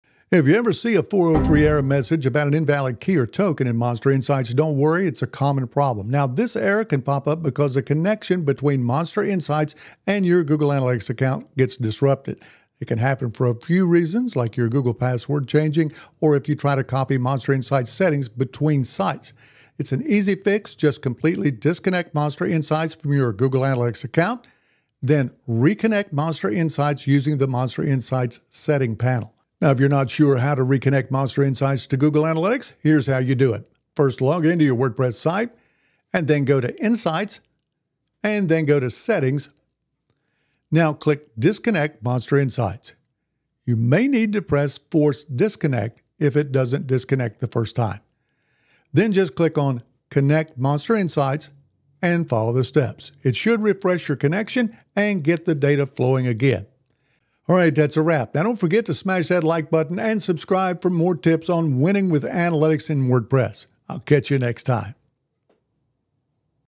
How to Fix MonsterInsights "403: The Key or Token is Invalid" Error
The MonsterInsights team shares how to easily fix the "Key or Token is Invalid" 403 error in MonsterInsights

Questions -
Have a question about using Google Analytics 4 for WordPress? Drop it in the comments!

About:
In this video, the MonsterInsights team goes over how to fix the 403 Error for an Invalid Key or Token in MonsterInsights.

Our mission here at MonsterInsights is to help WordPress website owners start making data-driven decisions to grow their business.